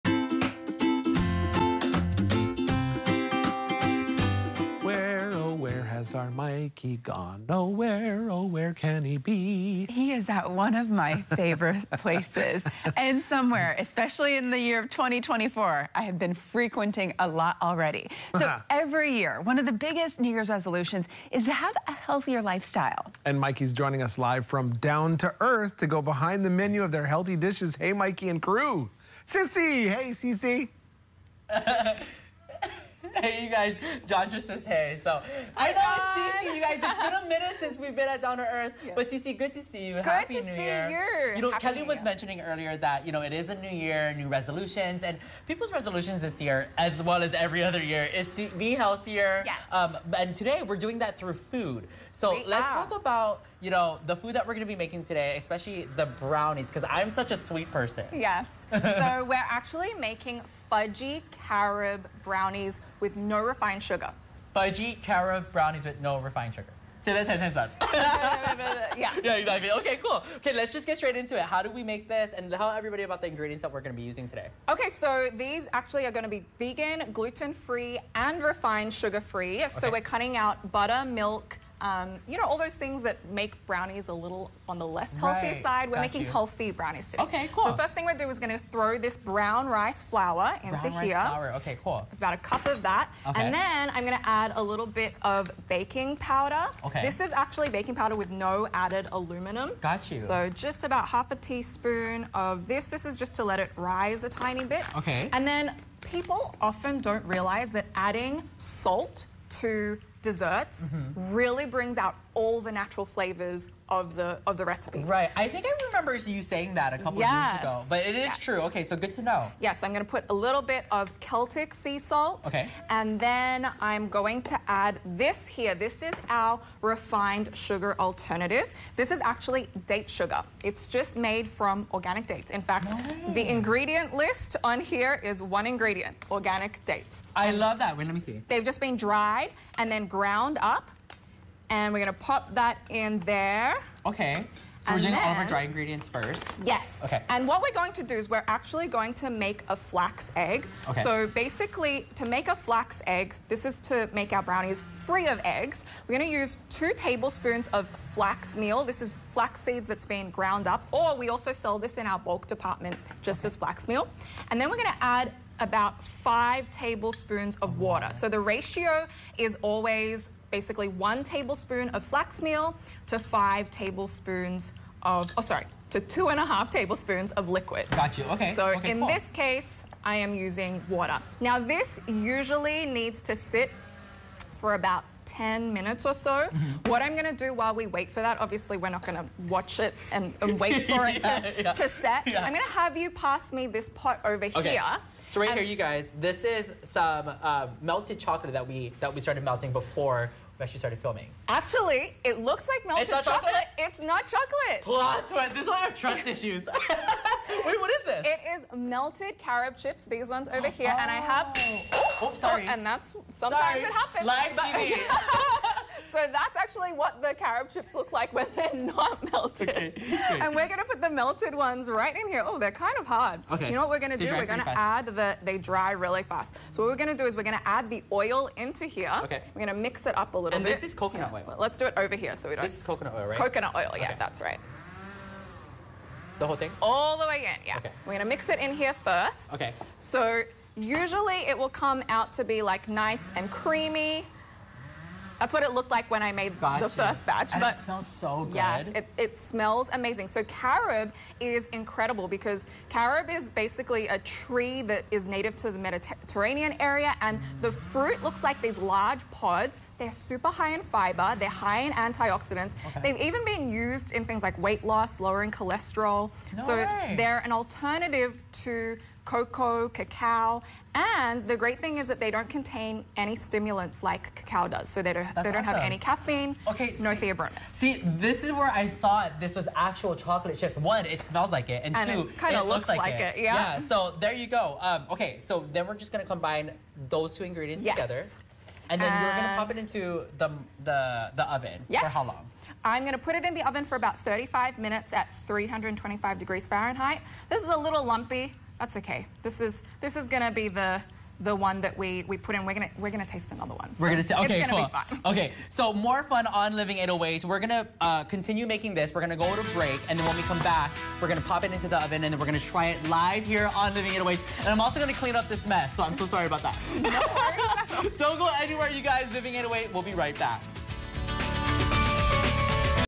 Eat This Brownie to Help With Your Overall Health - Part 1
Down to Earth provides ingredients and recipe for its Carob Brownies